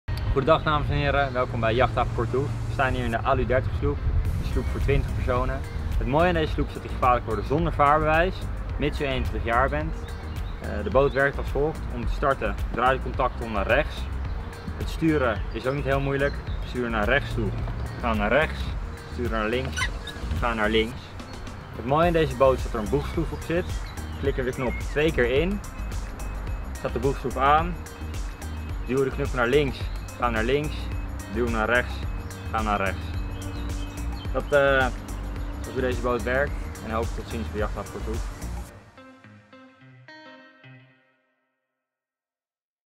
Instructie Allure 30 Jachthaven Kortenhoef
Heeft u onze Allure 30 sloep gereserveerd? Bekijk dan onze instructievideo!